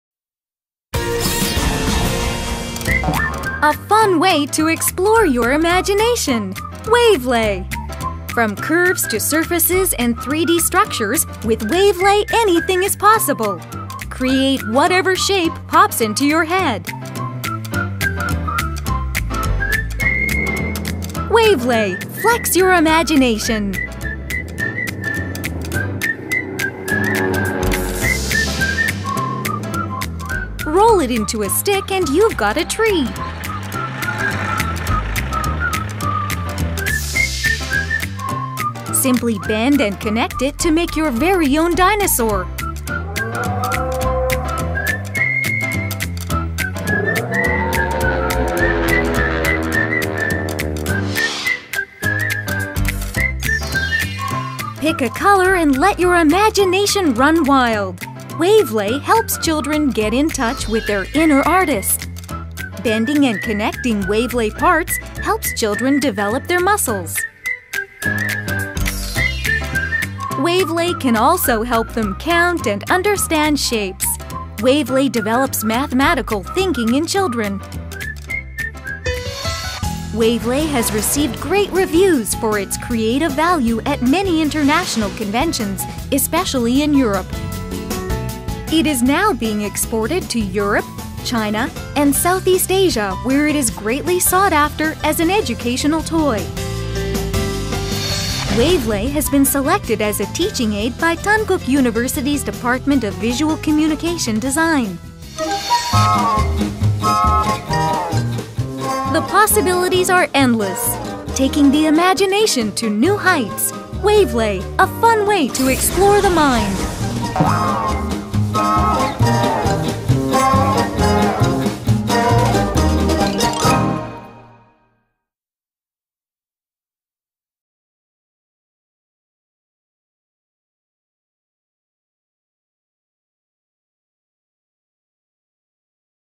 Гибкий конструктор Waveplay
Waveplay - гибкий конструктор нового поколения. Он позволяет создавать трехмерные фигуры, образуя линии и каркас из уникальных гибких палочек с крепежными элементами. Купить